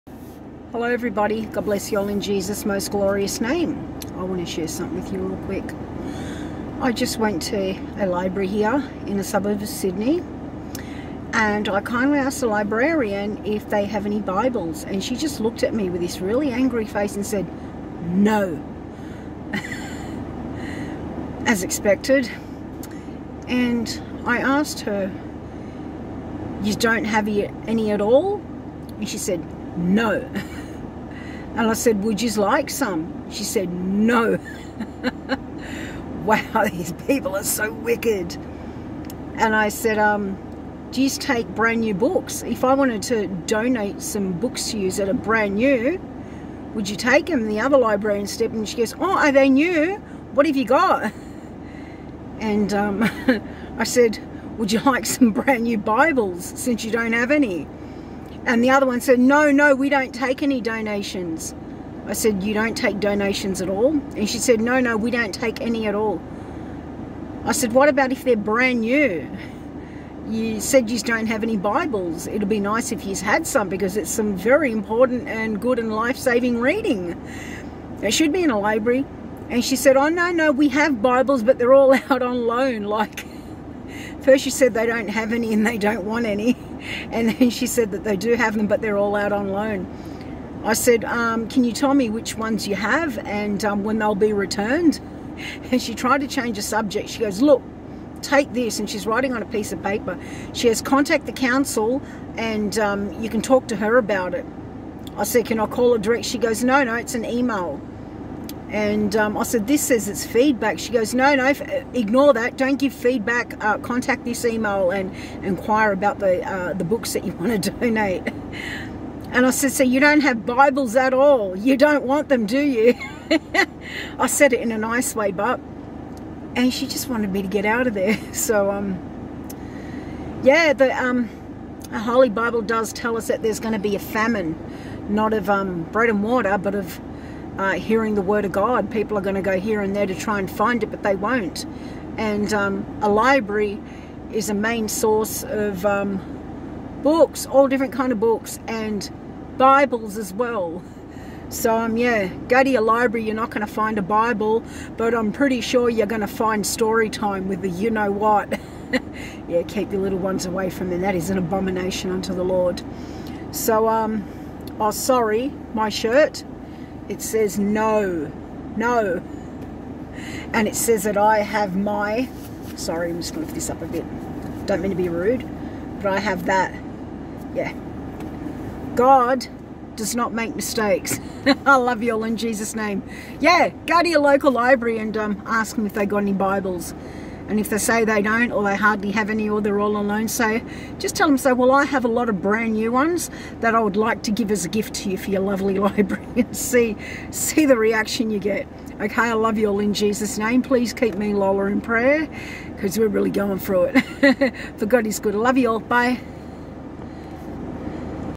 Sydney Library Hates The Holy Bible! Amos 8:11-12 Happening Right Now.
Amos 8:11-12

11" Behold, the days come, saith the Lord God, that I will send a famine in the land, not a famine of bread, nor a thirst for water, but of hearing the words of the Lord:
12 And they shall wander from sea to sea, and from the north even to the east, they shall run to and fro to seek the word of the Lord, and shall not find it."